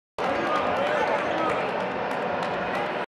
Facu Campazzo mostrando su habilidad con los pies en el MSG / Facu showing his skill with his feet
Facu Campazzo mostrando su habilidad con los pies / Facu showing his skill with his feet